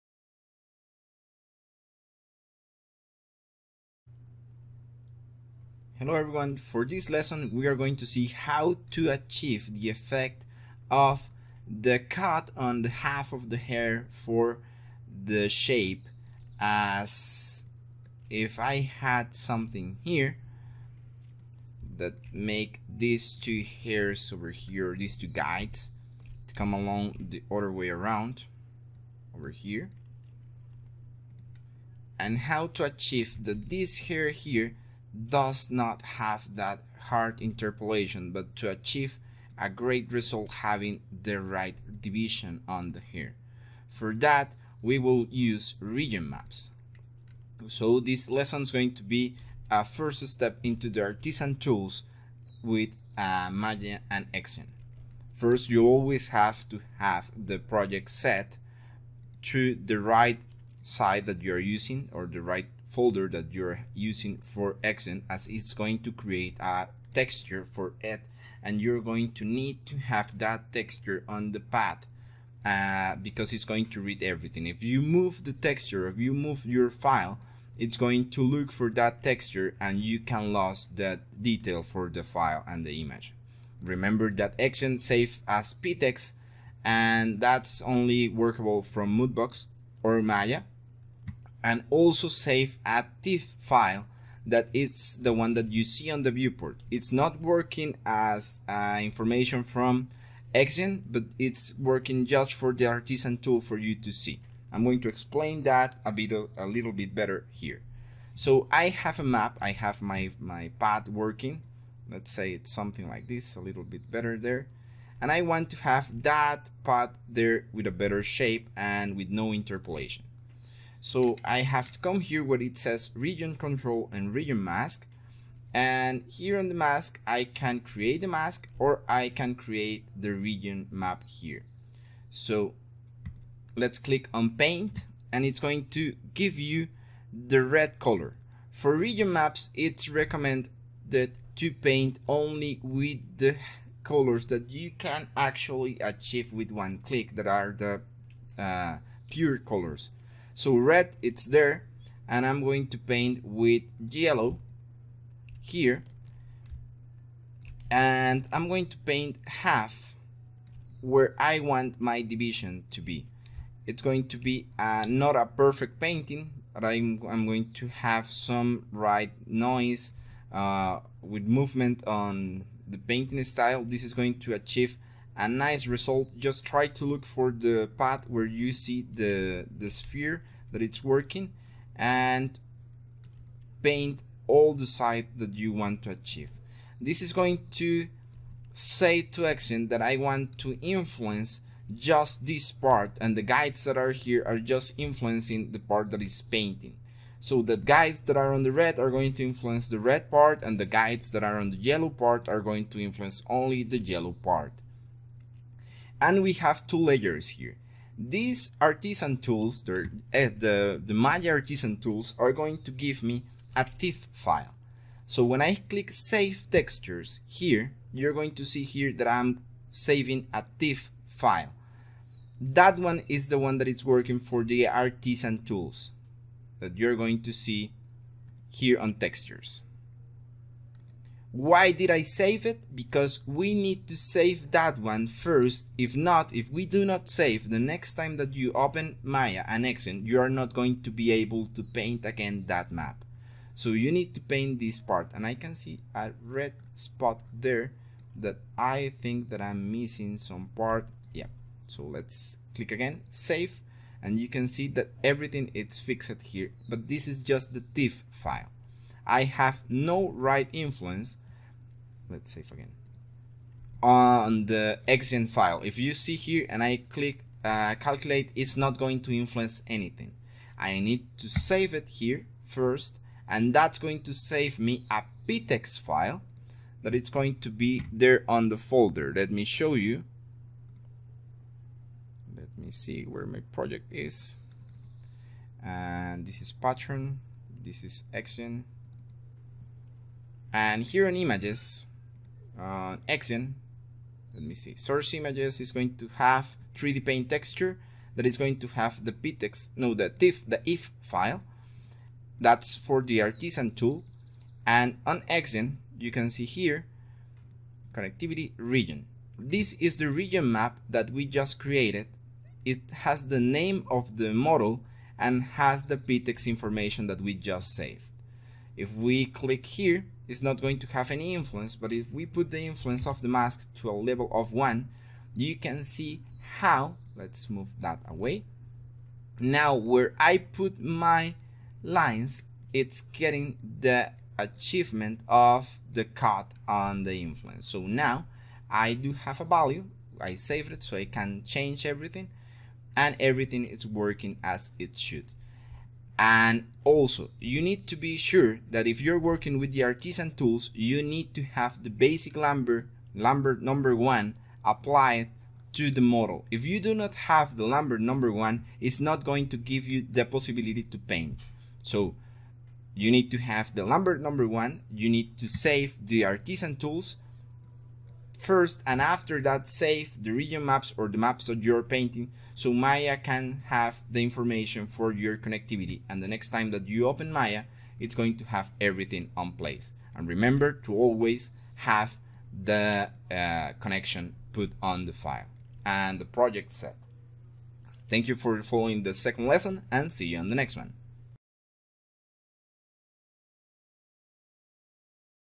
Introduction to Xgen: 02 Region maps and artisan tool
On this series you are going to learn the basics of xgen grooming completely free.

A lot of tips and tricks on how to work with xgen and solve most of the common issues that you can find when working with it. I will try to make as much videos as possible for this series.

As you may have notice, xgen can be quite tricky sometimes with random errors that most of the time are because of the wrong use of the workflow, but once you get it right, you would notice how strong is the software that Maya has and the possibilities that you can open to get a better hair on your works!

I want to thank to all the patrons that made this series possible.